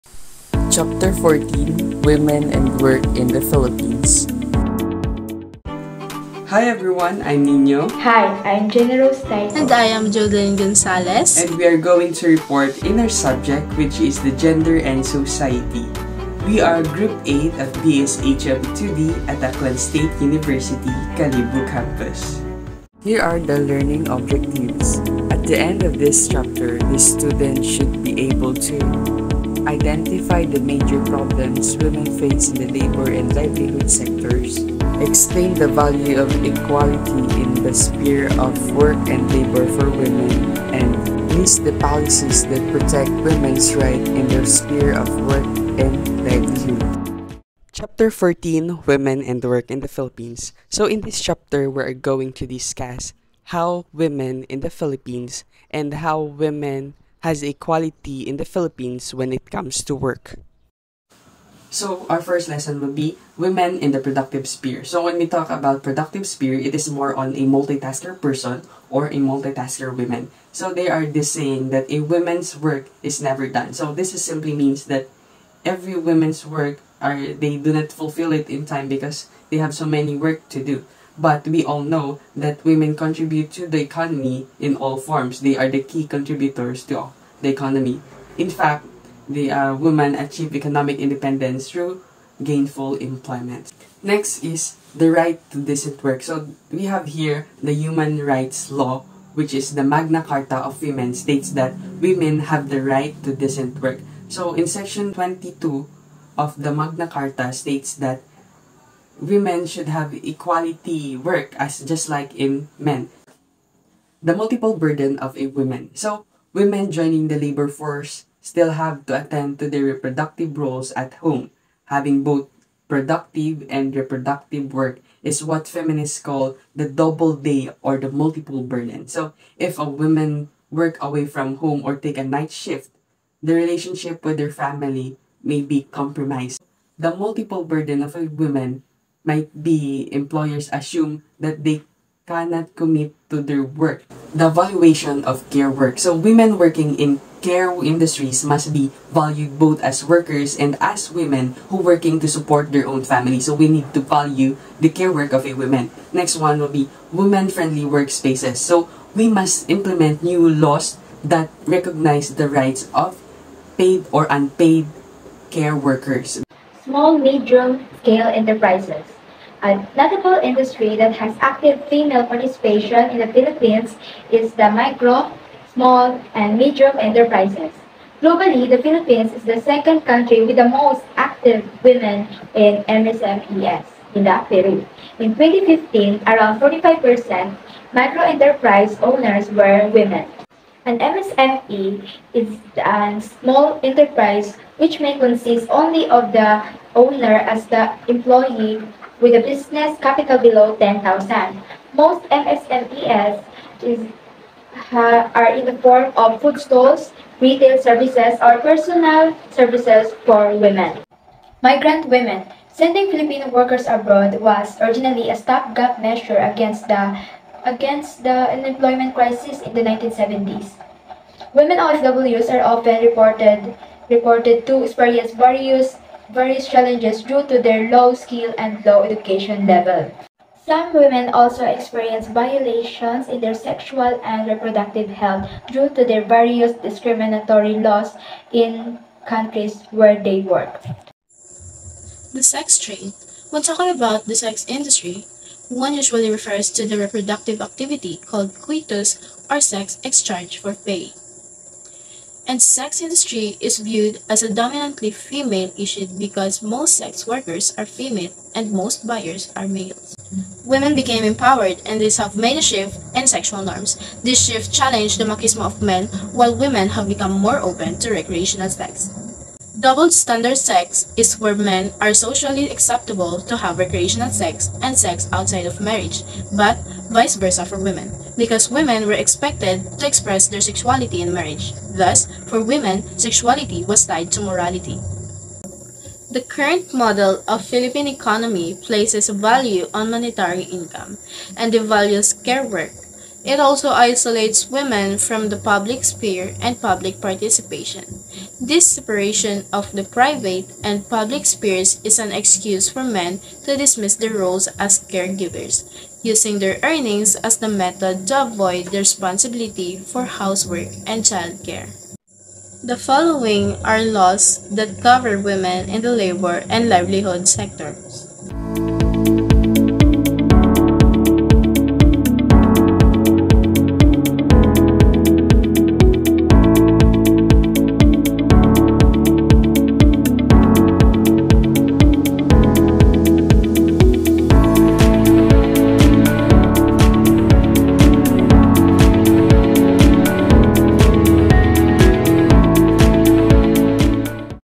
EGS - Chapter 14 Women and Work in the Philippines - BSHM 2D 2023-2024
This video focuses on making students be able to identify the major problems women face in the labor and livelihood sectors; explain the value of equality in the sphere of work and labor for women; and list the policies that protect women's rights in work and livelihood.

Guide Questions for Study
1. What may be considered the unique contributions of women to economic development?

2. What are some of the major issues affecting women in the sphere of work and labor? How does the Philippine government address these problems?

3. How can you explain the statement "To achieve true gender justice in a society, the valuation of gendered tasks must be given equal importance" ?

4. What makes women in the informal sector especially vulnerable?

5. Name the existing labor policies in the Philippines aiming to promote the rights of women in the workplace?

References:
Rodriguez, A.M. and Rodriguez, A.M.L., 2019. Gender and Society.

Students, log in in your gmail account first before viewing the video and commenting. Indicate your full name and section. Thanks.